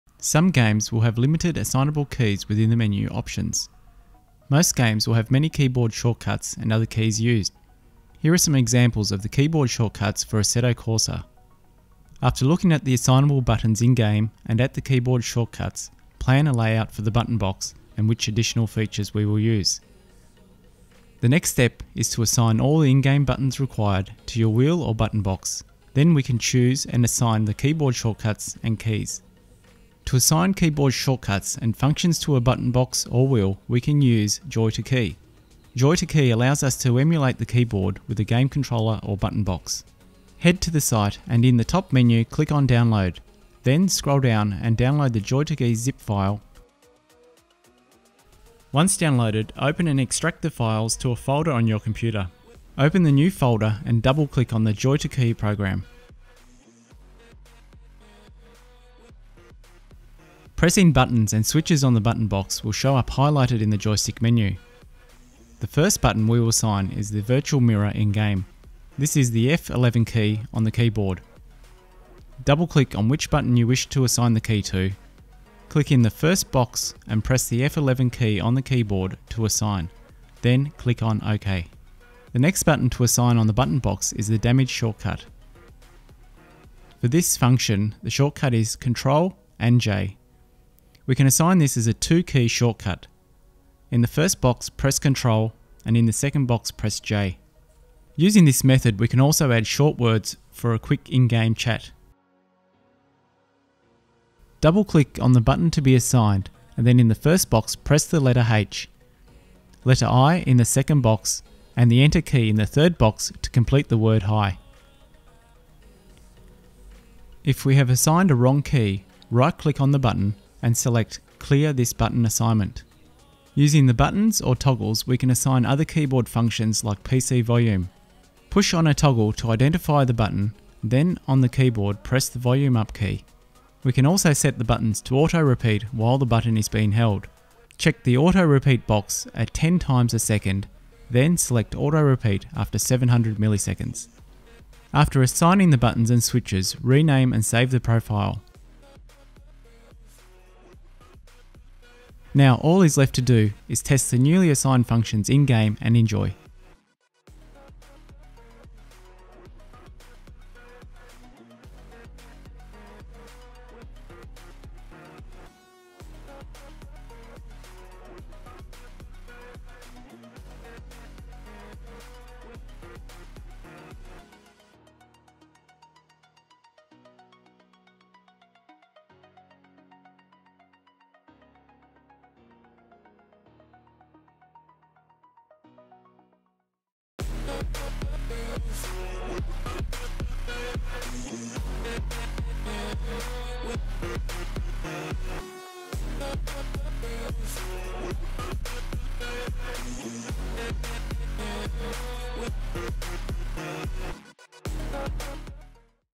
HOW TO ASSIGN BUTTON BOX TO KEYBOARD FUNCTIONS
How to assign Keyboard functions to Button box with JoyToKey An easy tutorial to setup your Sim racing simulator.

If you know someone interested in this Video please share it with them.